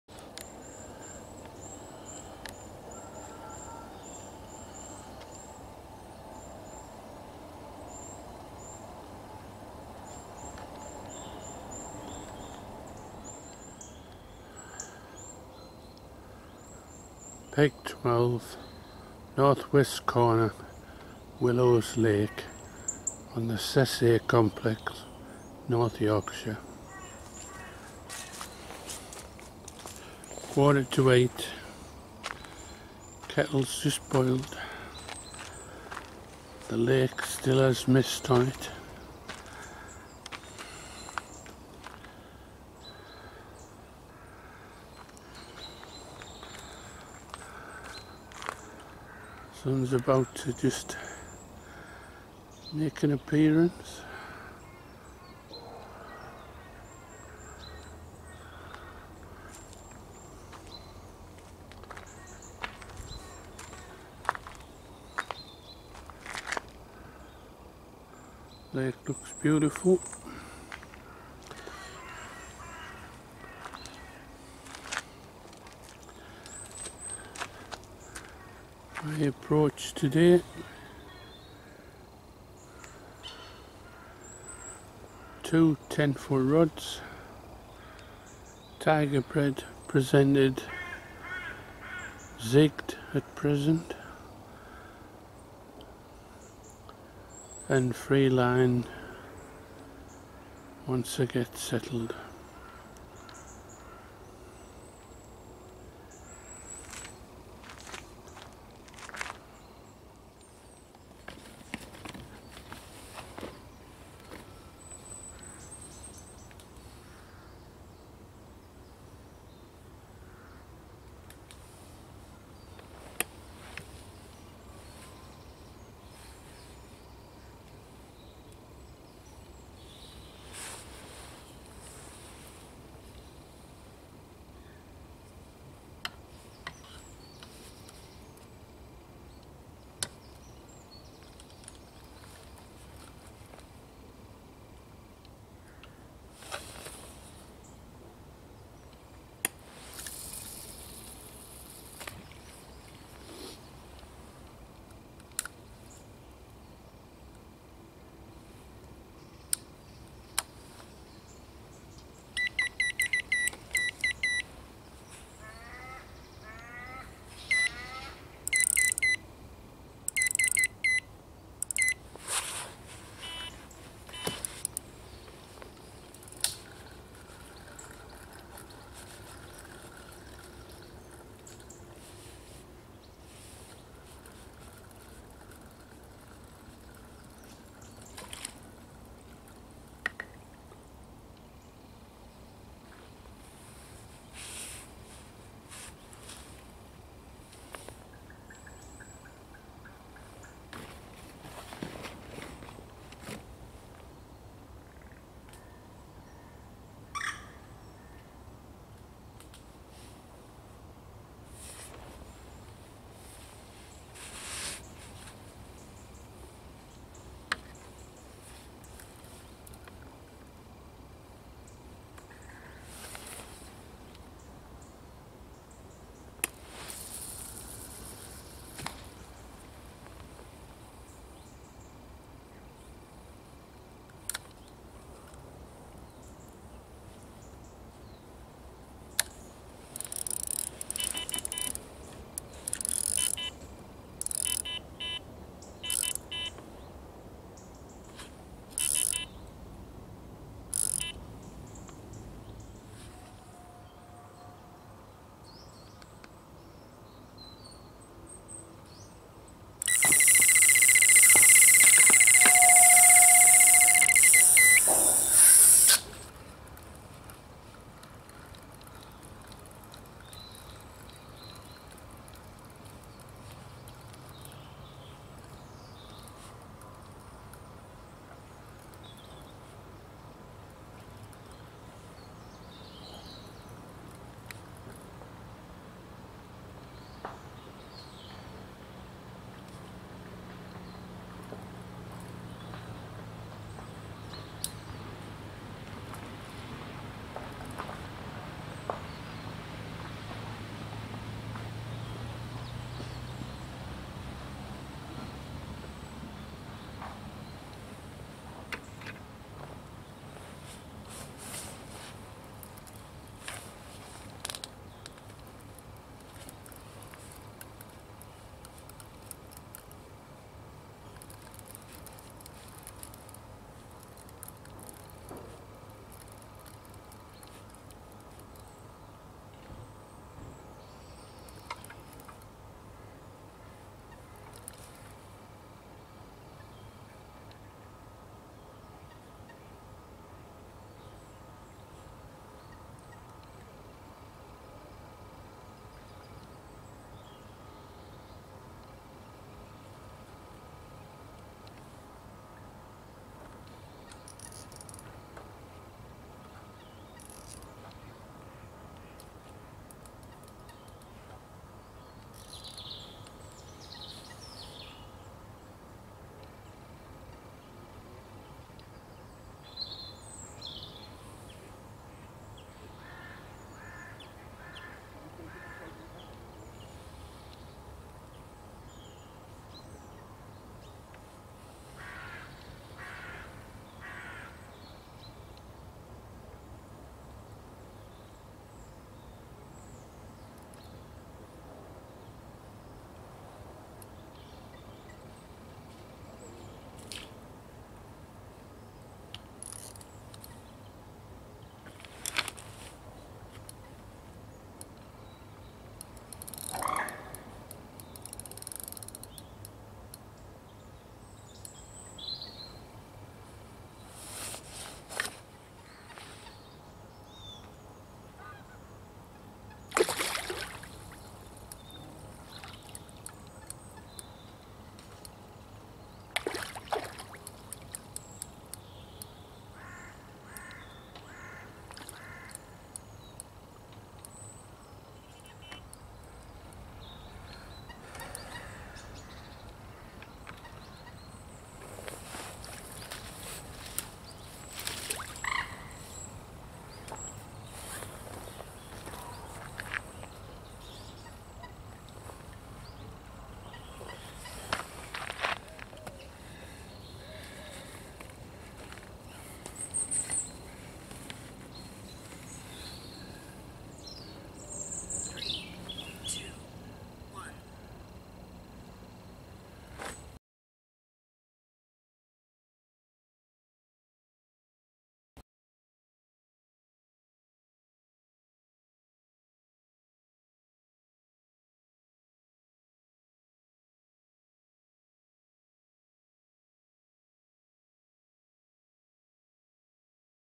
8am carp Willows Lake The Oaks Oct 1st 2018
First day of October at Willows Lake, The Oaks, Sessay, North Yorks.